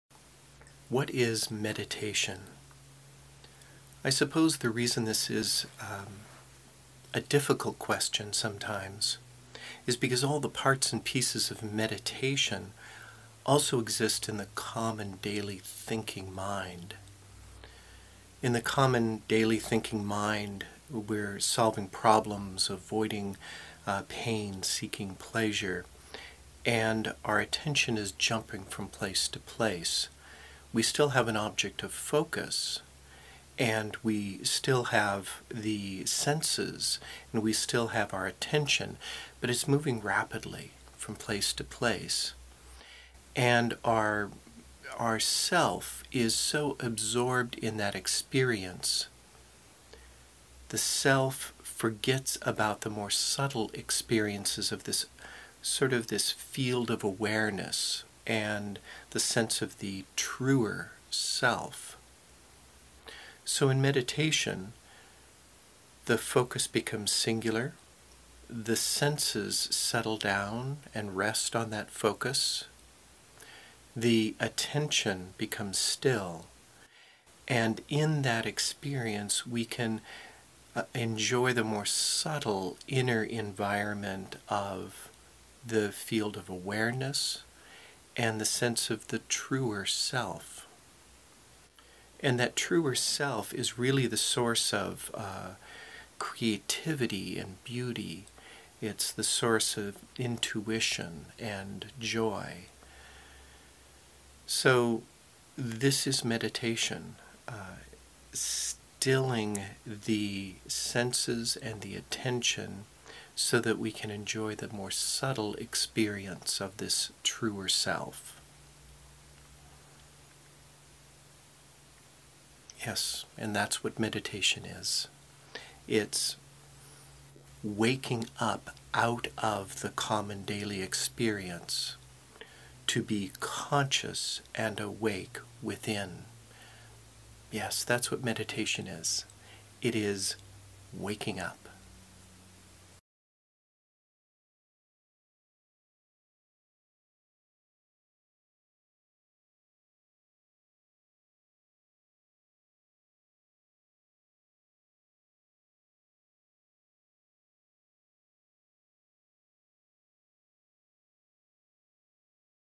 What is Meditation
This is a creative yet concise answer to the question, what is meditation?

Quiet Mind Cafe offers meditation, visualization, relaxation for health and wellbeing, creativity and insight. Here you will find an in-depth exploration of the meditative and imaginal experience.